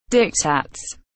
How to pronounce Diktats / Diktats pronunciation
Raccoon teaches you how to pronounce Diktats . Raccoon vous apprends a prononcer Diktats . Como pronunciar Diktats